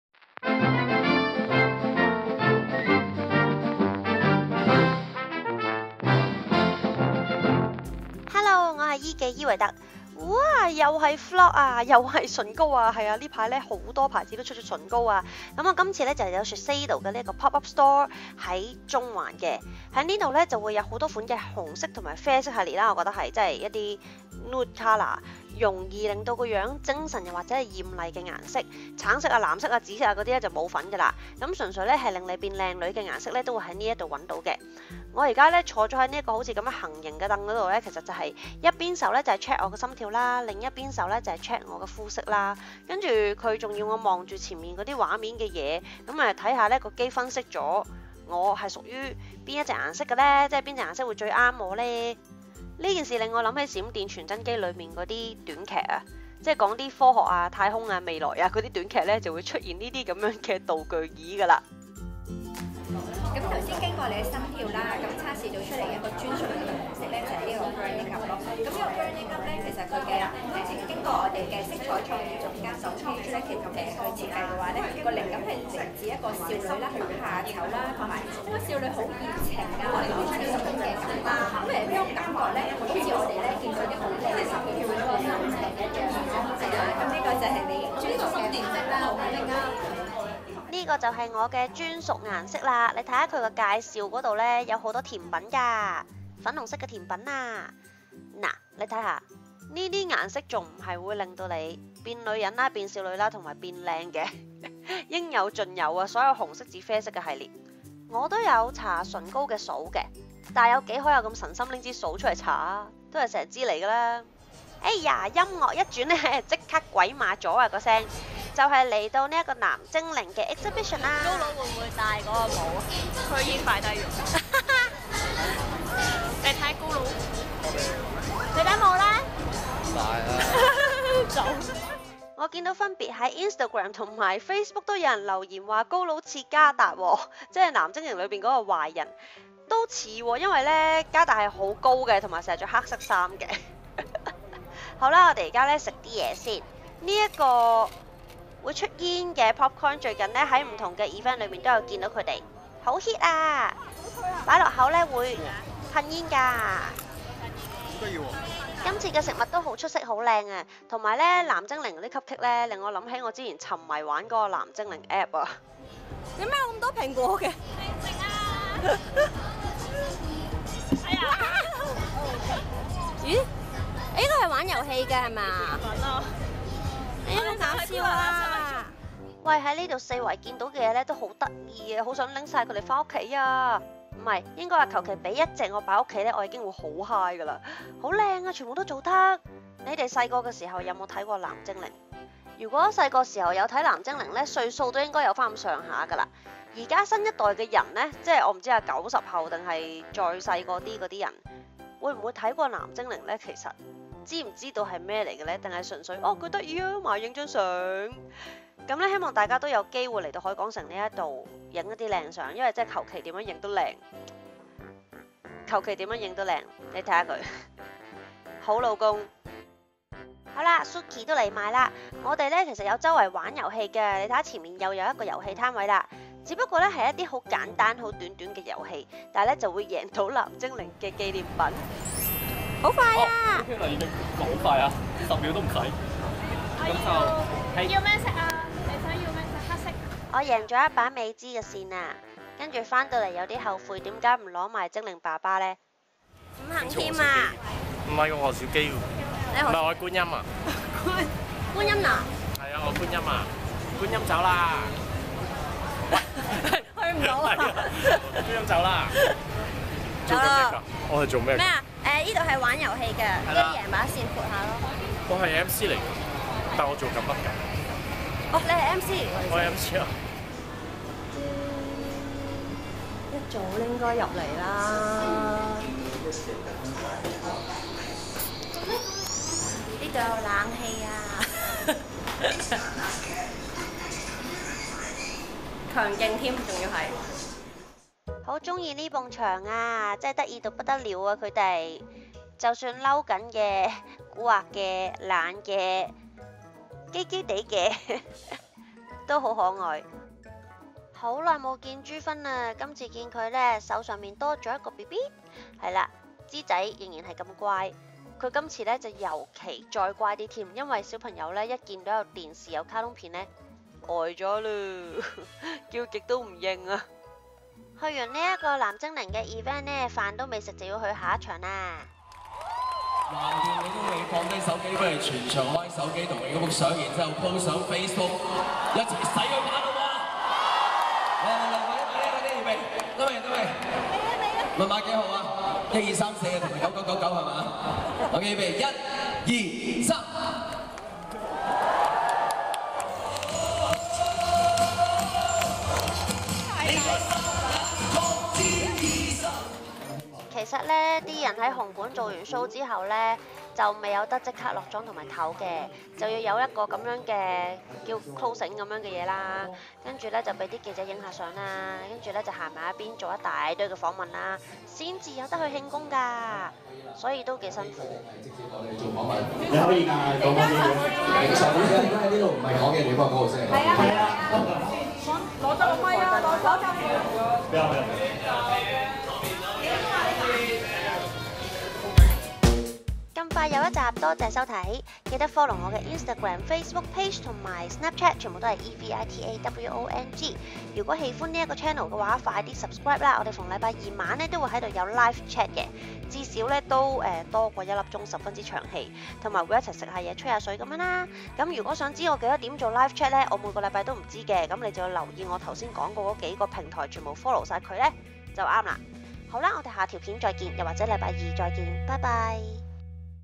EKEE - Red Smurf Jan Lamb//伊維特
I am a lucky girl!! fun day at shiseido pop up store at central, at Surfs Party at the harbour city, and of course big bro Jan Lamb's talk show. 捐獻字幕吧！WELCOME to submit subtitles to this video!! thank you very much if you are willing to do so!! i will put your name in this info box to THANK you!!

EVITAWONG